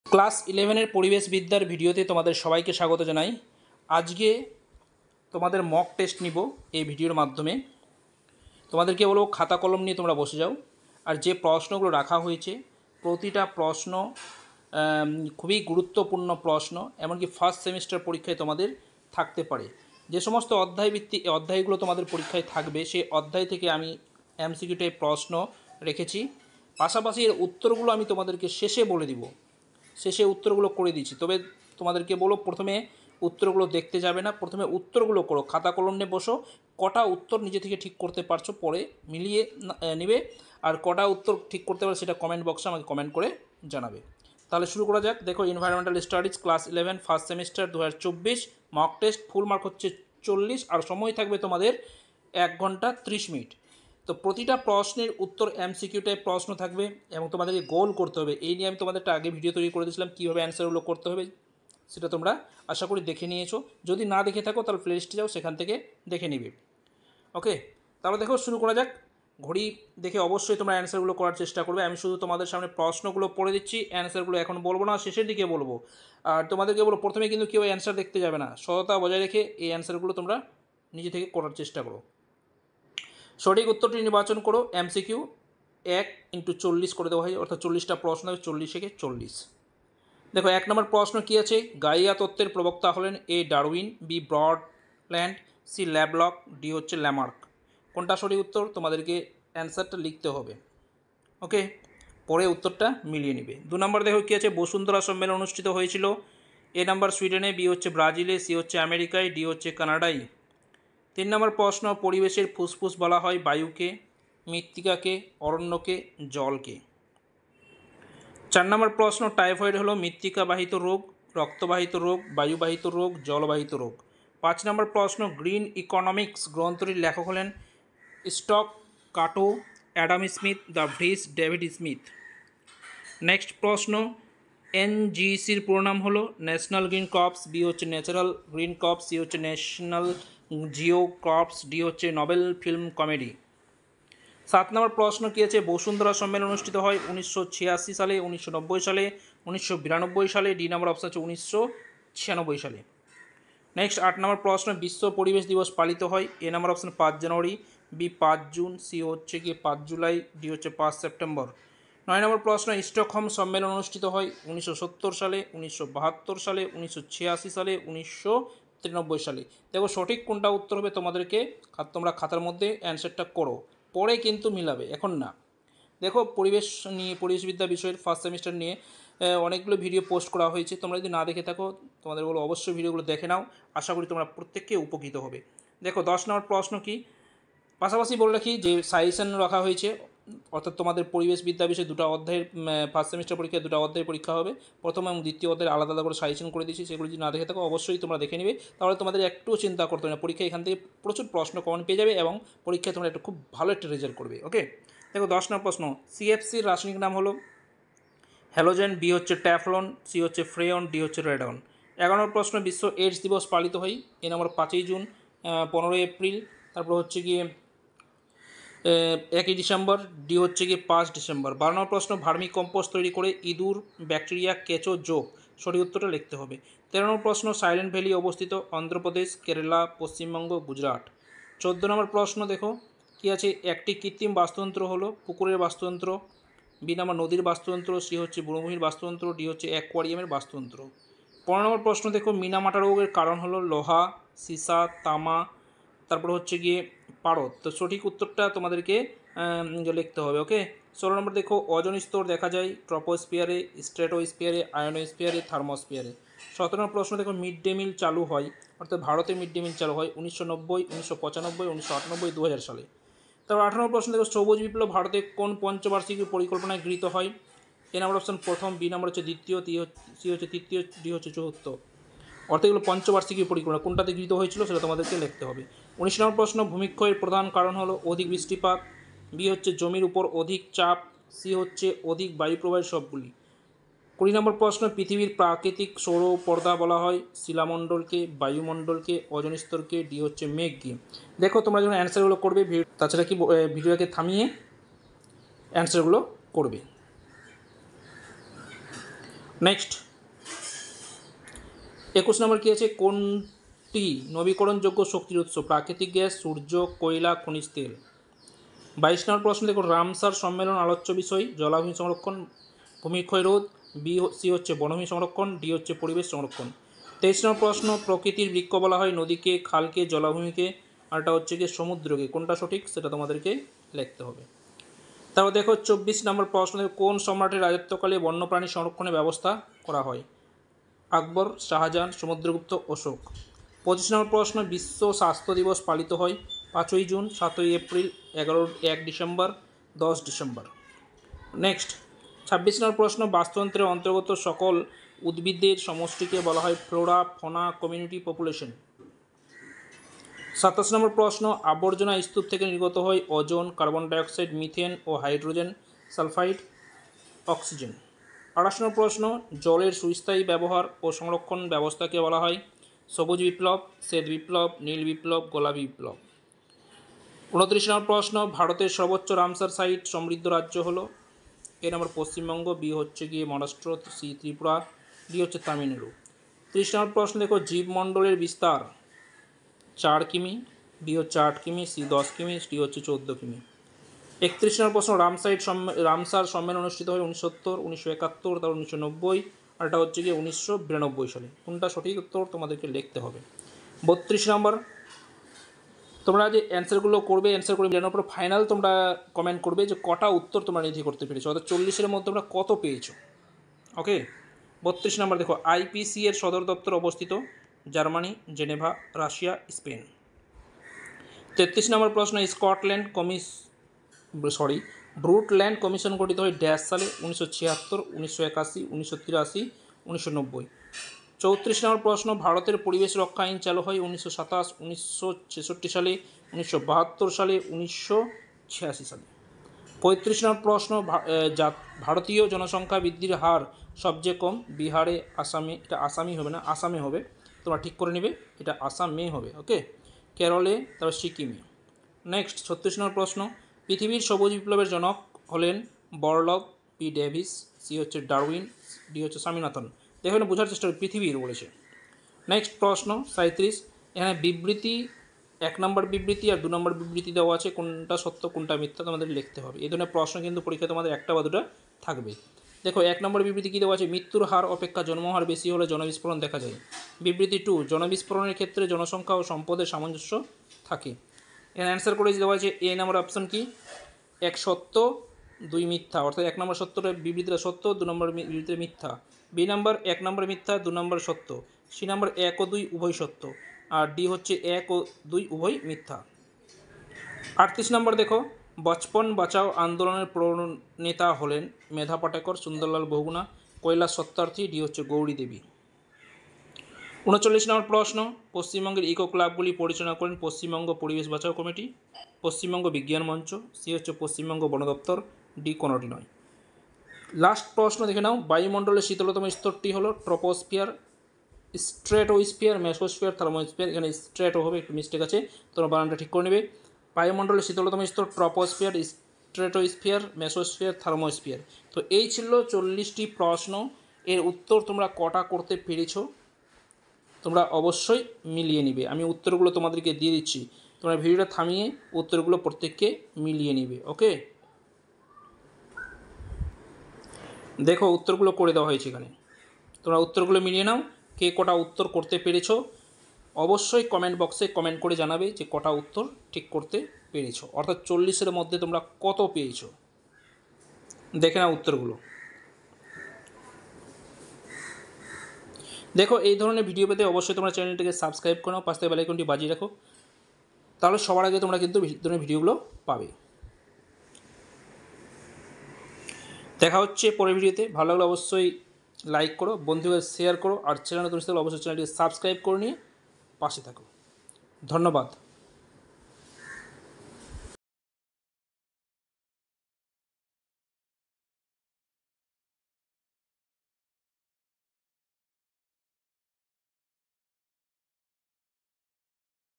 Class 11 First Semester Environmental Studies Mock Test//Envs Mock Test//Envs Question 2024
এই ভিডিওর মাধ্যমে ক্লাস ১১ ফার্স্ট  সেমিষ্টার পরিবেশবিদ্যা বিষয়ের মক টেস্ট নেওয়া হচ্ছে। তোমরা কতগুলো অ্যান্সার করতে পারছ অবশ্যই কমেন্ট বক্সে জানাবে। প্রতিটি প্রশ্নের উত্তর ভিডিওর শেষে করে দেওয়া হয়েছে। উত্তরগুলো মিলিয়ে নাও।